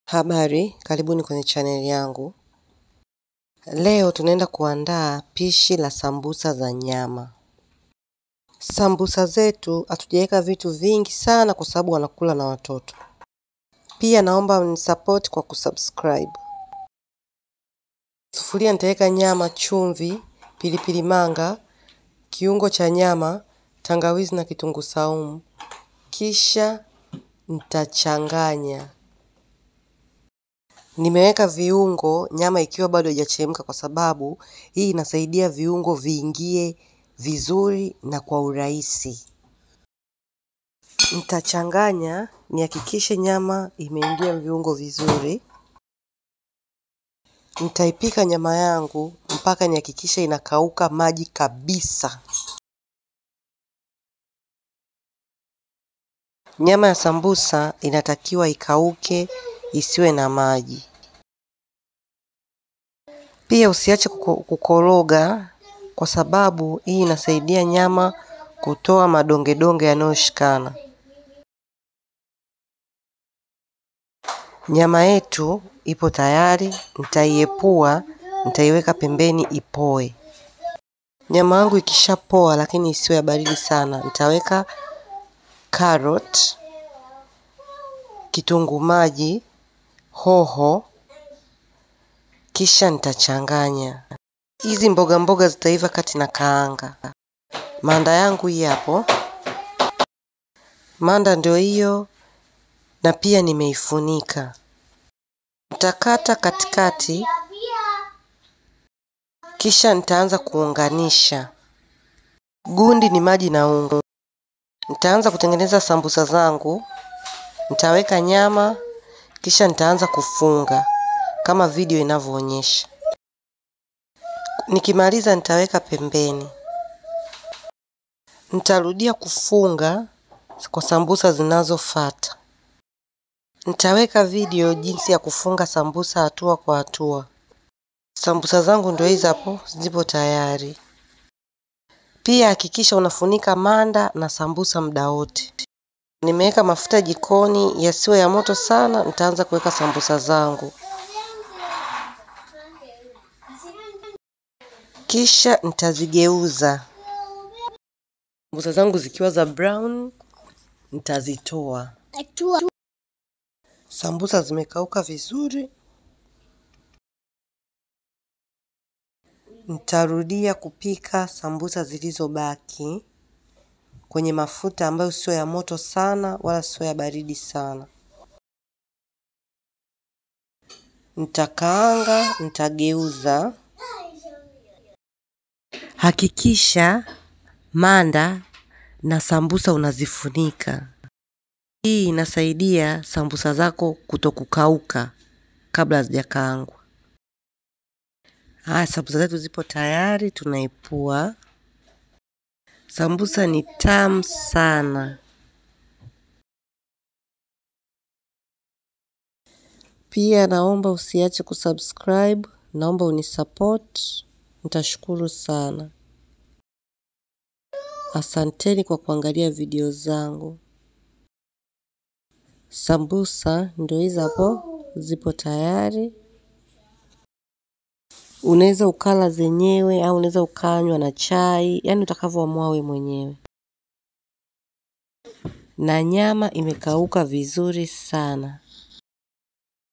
Jinsi Ya Kupika Sambusa Za Nyama
Manda
Nyama Ya kusaga 1 kg
Karoti 1 kubwa
Vitunguu 2
Pilipili hoho 1
Pilipili manga kijiko 1cha  chai
Chumvi kijiko 1 cha chai
Binzari nyembamba kijiko 1 cha chai
Tangawizi kitunguu saumu paste kijiko 1 cha chakula
Mafuta ya kukaangia
Kiungo Cha nyama au Royco kijiko 1 cha chai